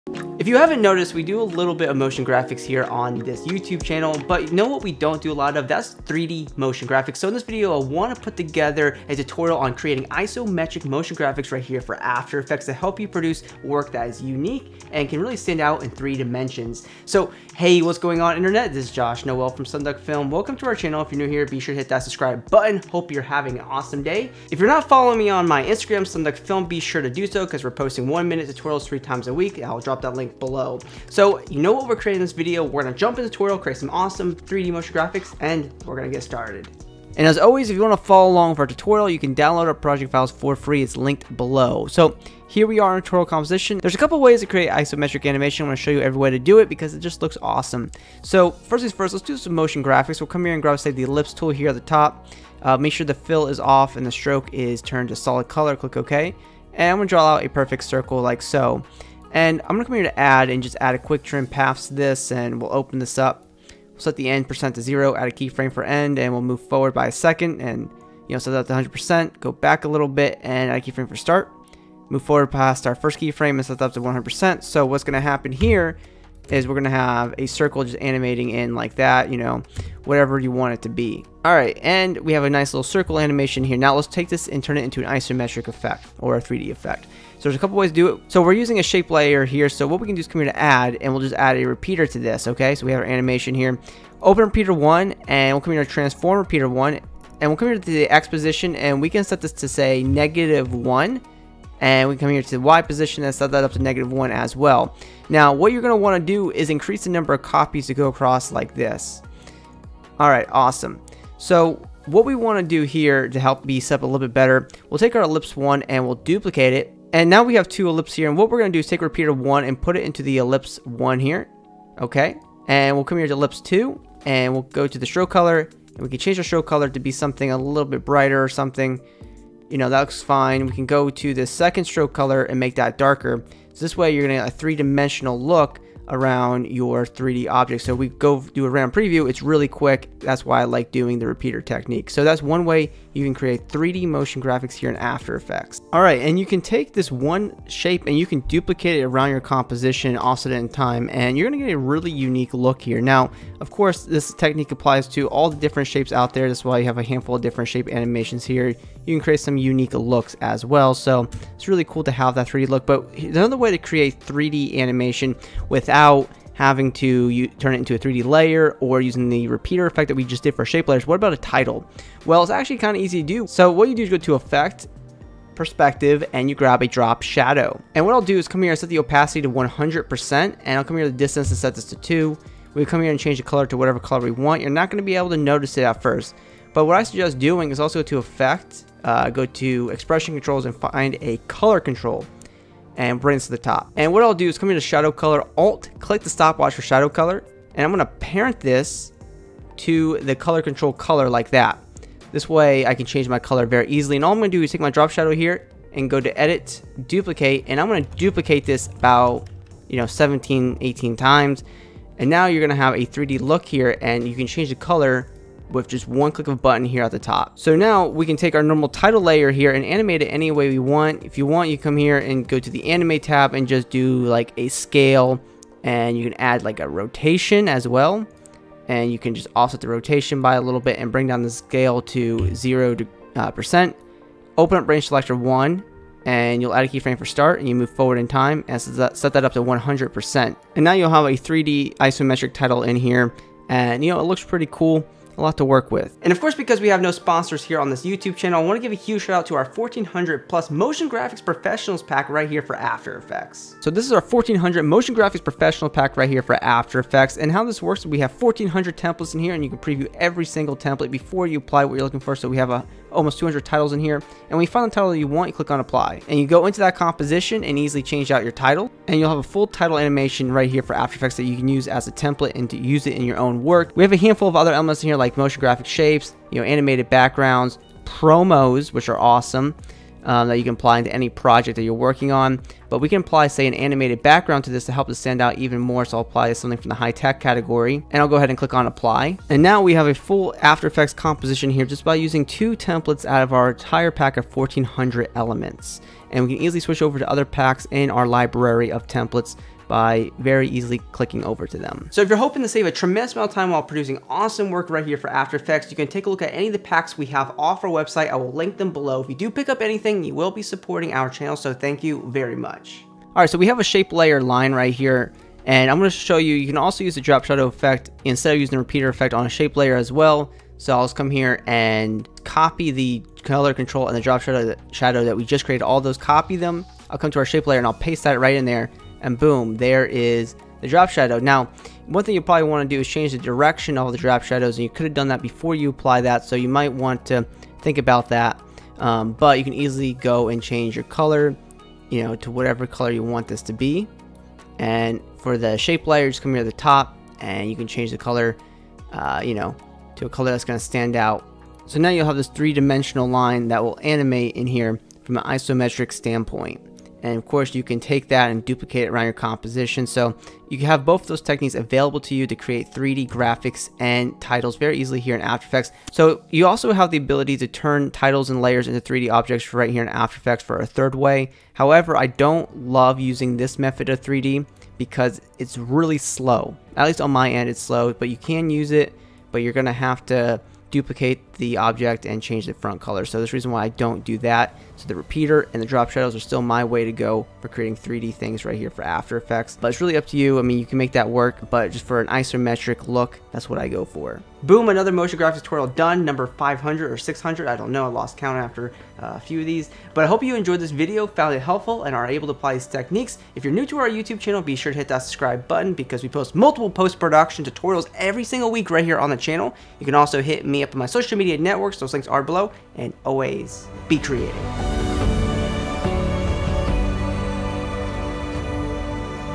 3 Isometric/3D Motion Graphics Techniques in After Effects
Isometric is a simple concept of a 3D look. In this After Effects tutorial, we’re going to jump into a fun and easy tutorial for taking your motion graphics and turning them into 3D motion graphics. 3D motion graphics and titles allow you to create work that pops and stands out. The three concepts we’re going through in this After Effects tutorial are for shape layers and titles.

 Over 1400+ elements for After Effects from titles, backgrounds, motion graphics, promos, effects, and transitions!

Timecode:
0:00 Intro – Smash The Like Button :)
0:37 Technique 1 - Isometric Shapes
2:25 Technique 2 - Isometric Titles
4:06 1400+ Motion Graphics Pro Pack
5:13 Technique 3 - Drop Shadow For Isometric
6:34 Outro - Please Subscribe :)